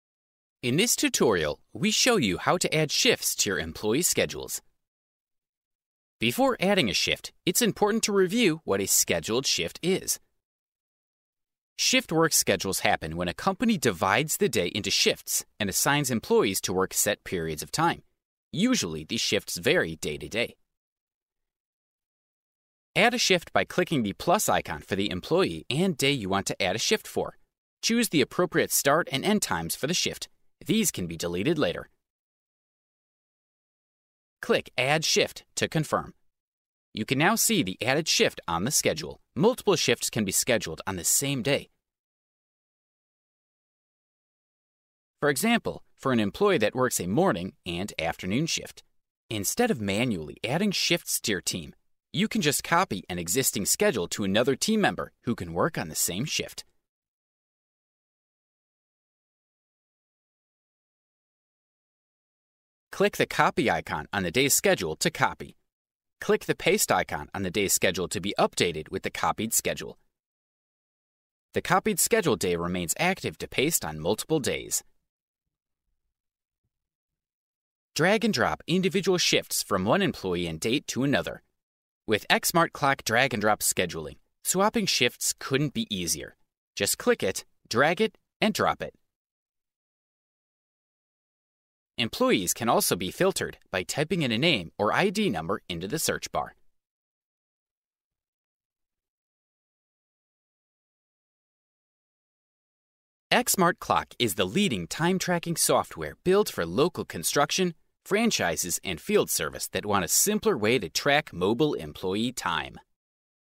Adding shifts to your employee schedules - XmartClock
In this tutorial we show you how to add shifts to your employee schedules.
Before adding a shift, it's important to review what a Scheduled Shift is.
Shift work schedules happen when a company divides the day into shifts and assigns employees to work set periods of time. Usually these shifts vary day to day.
Add a shift by clicking the plus icon for the employee and day you want to add a shift for.
Choose the appropriate start and end times for the shift. These can be deleted later.
Click 'Add Shift' to confirm.
You can now see the added shift on the schedule.
Multiple Shifts can be scheduled on the same day, for example for an employee that works a morning and afternoon shift.

Instead of manually adding shifts to your team, you can just copy an existing schedule to another team member who can work on the same shift.
Click the Copy icon on the day’s schedule to copy.
Click the Paste icon on the day’s schedule to be updated with the copied schedule.
The copied schedule day remains active to paste on multiple days.

Drag and drop individual shifts from one employee and date to another.
With XmartClock drag & drop scheduling, swapping shifts couldn't be easier. Just click it, drag it and drop it.

Employees can also be filtered by typing in a name or ID number into the Search bar.

XmartClock is the leading time tracking software built for local construction, franchises and field service that want a simpler way to track mobile employee time.